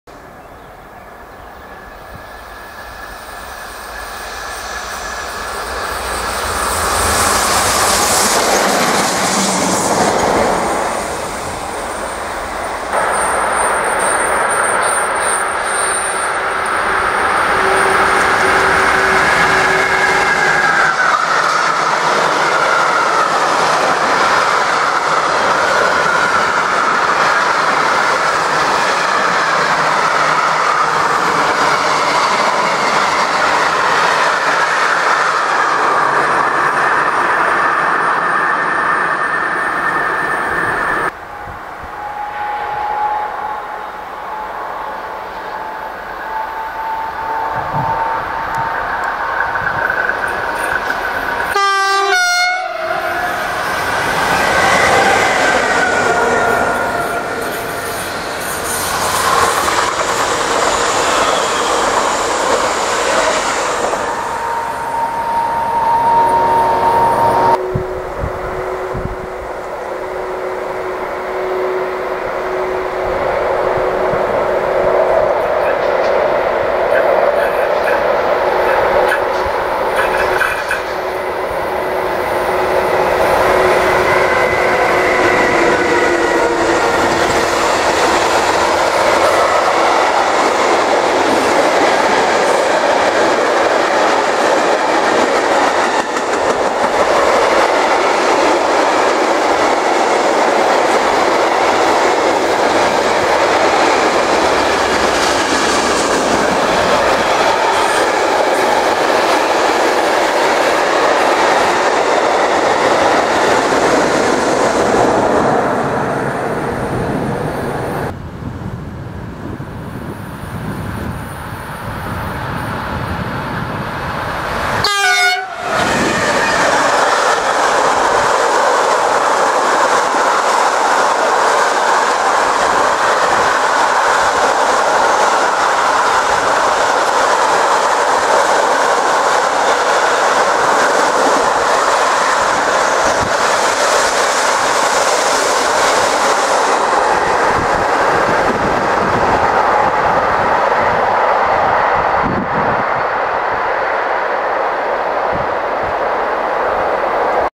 103 222, Akiem E 37 mit Makro, Parallelfahrt mit Hamster uvm. am 30.06.2013 im Frankenwald
Frankenwaldbahn:
Am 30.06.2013 ging es am Nachmittag an die Frankenwaldbahn. Grund hierfür war die vorgemeldete 103 222, die von Leipzig nach München unterwegs war. Leider kamen wir zu spät und mussten diese in Förtschendorf aufnehmen. Danach noch kurz nach Pressig geschaut und dann ab an die Frankenwaldrampe. Dort kam gleich ein seltenes Highlight und zwar die Überholung eines Regionalexpresszuges durch die Akiem E 37 / 37 531 kurz nach Förtschendorf. Diese war auf dem Weg nach Steinbach am Wald um dort den Wiegand-Glas Zug abzuholen und diesen Richtung Frankreich zu ziehen. welcher auch wenig später im Video zu sehen ist.

Zu sehen ist:
- BR 103 / 103 222 als Tfzf in Förtschendorf gen Kronach
- BR 152 DB Schenker mit gemischten Güterzug in Pressig gen Steinbach am Wald
- Akiem E 37 / 37 531 als Tfzf in Fötschendorf gen Steinbach am Wald
- ET 442 / Talent 2 als RE gen Steinbach am Wald
- BR 152 DB Schenker mit Lkw-Walter Klv und BR 151 als Schublok gen Steinbach
- Akiem E 37 / 37 531 mit H-Wagenzug gen Kronach

Ich hoffe das Video gefällt euch und über Kommentare / Bewertungen würde ich mich sehr freuen :-)